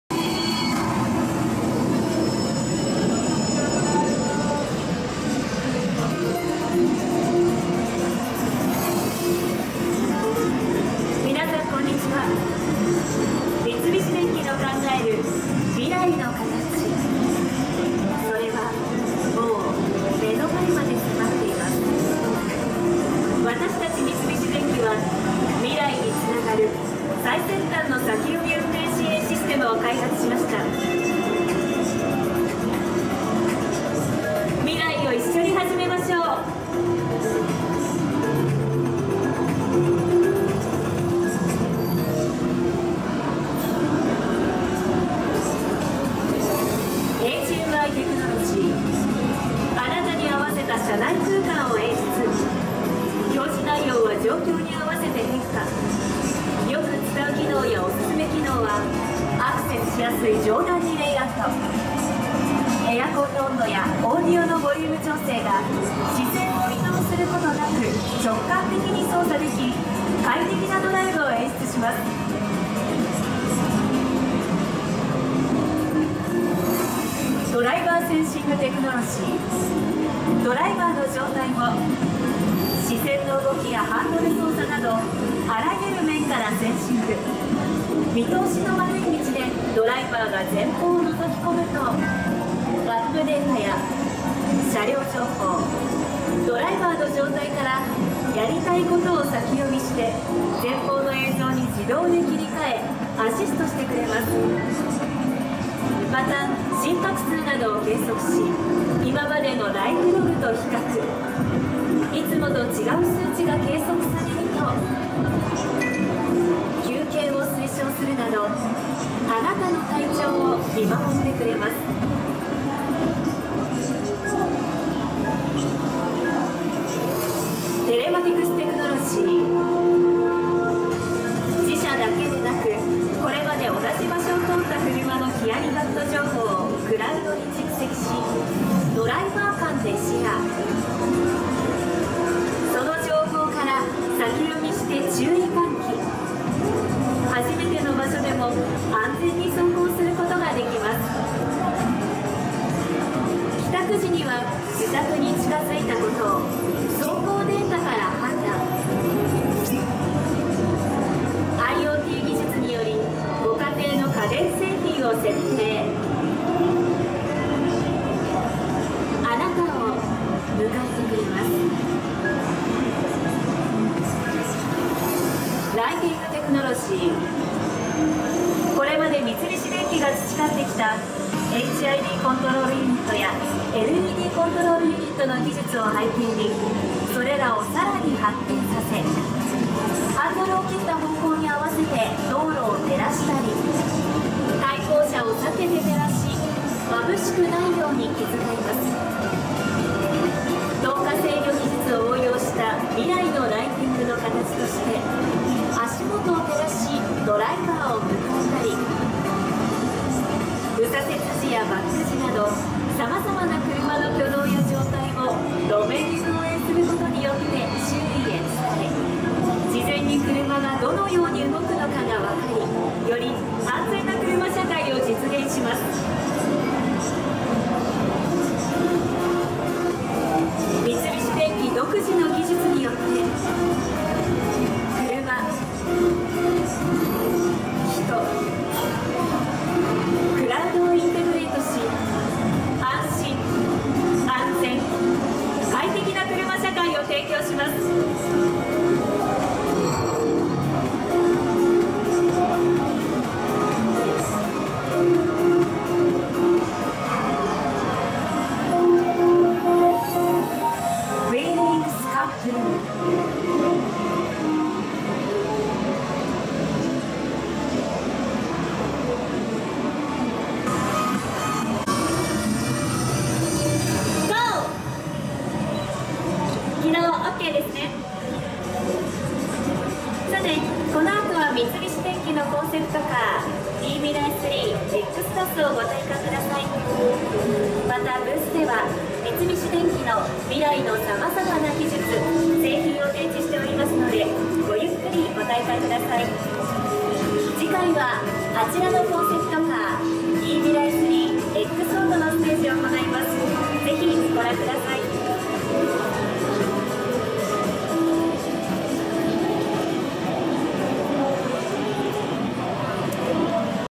東京モーターショー２０１５コンパニオン 三菱電機
第44回東京モーターショー三菱電機ブースでのライブプレゼンテーションの様子です。2タイプのコンセプトカーで予防安全・自動運転技術・高度安全支援技術を提案しました。